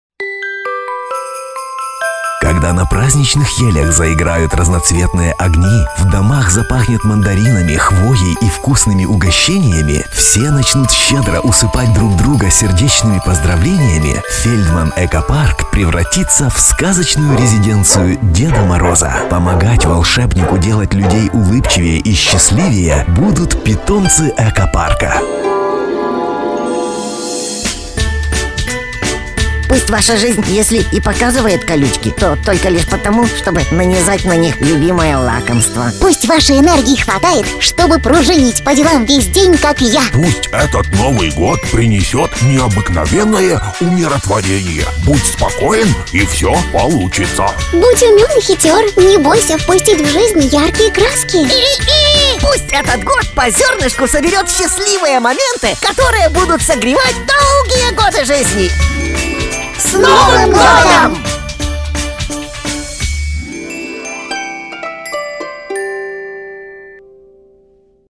Фельдман Экопарк поздравляет с Новым 2017-м годом!
Фельдман Экопарк поздравляет всех с Новым 2017-м годом! Мы желаем, чтобы год Огненного Петуха непременно порадовал вас яркими событиями, теплыми встречами и полезными открытиями. Принимайте самые теплые поздравления от четвероногих друзей Экопарка – внимание на экран!